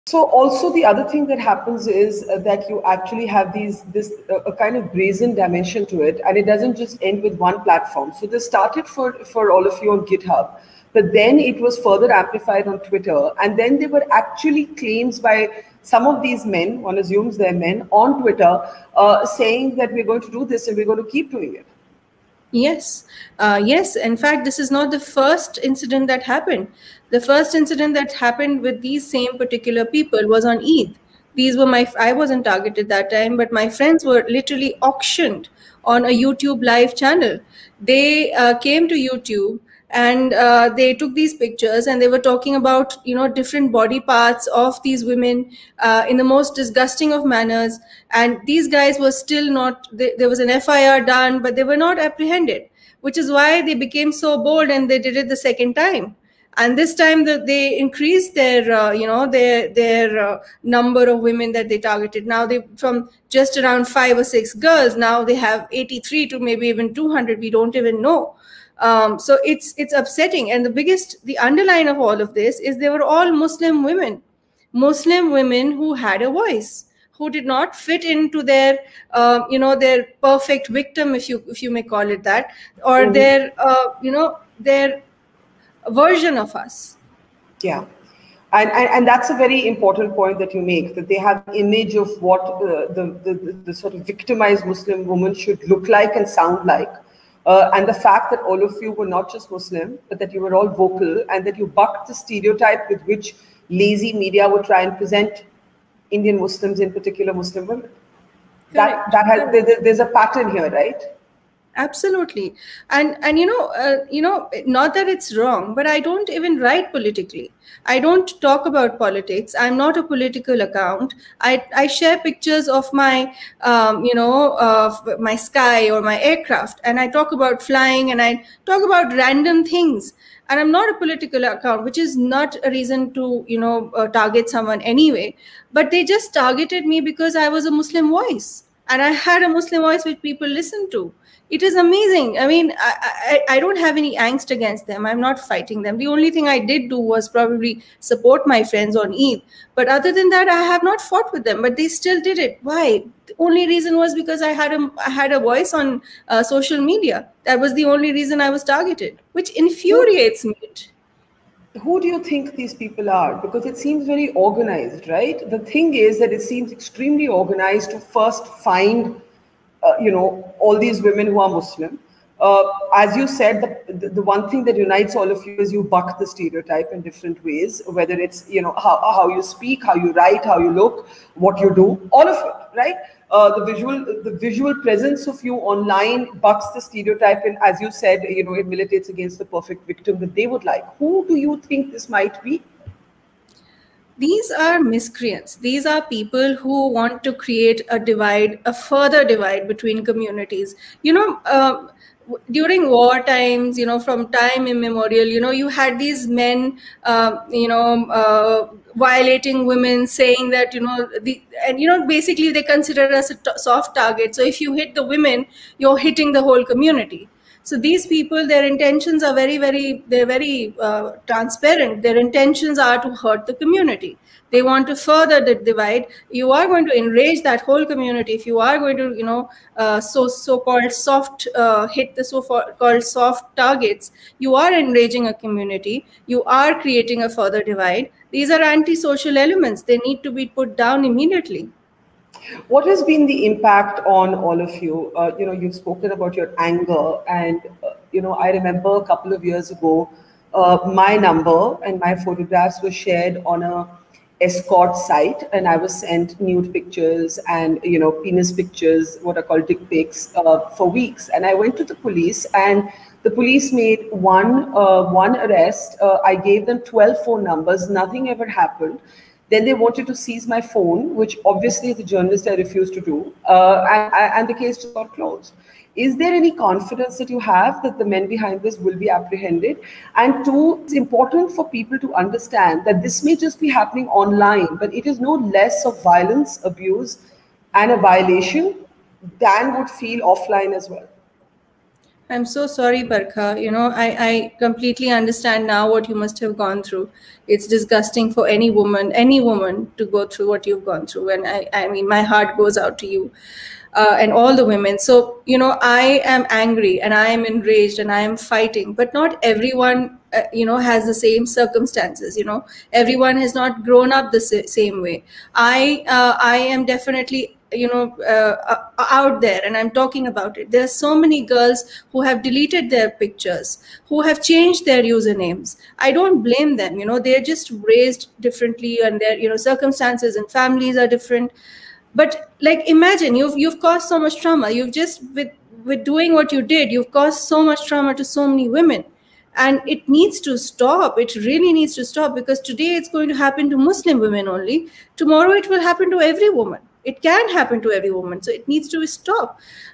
"I Was Targeted Only Because Of My Religion & Gender" | Muslim Woman Who Was Listed On 'Sulli Deals'
Hana Khan, a commercial pilot who was also listed on the application "Sulli Deals" told Mojo Story that this has not happened for the first time. "In last Eid, many Muslim women were auctioned on a youtube live channel. FIR was registered last time but no one was apprehended and so they became more confident", she says. Hana says that she has a non political account nevertheless she was targeted just because of her gender and her religion that's muslim.